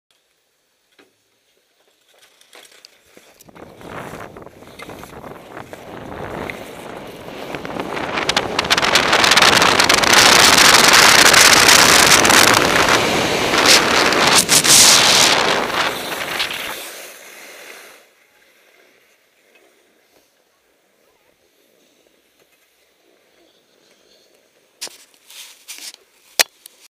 0-81kmh (0-50mph) in under 10 Seconds with ski [HD]
nearly like a car. in under 10 sec to 50mph with ski.

Shot with a ContourHD 1080p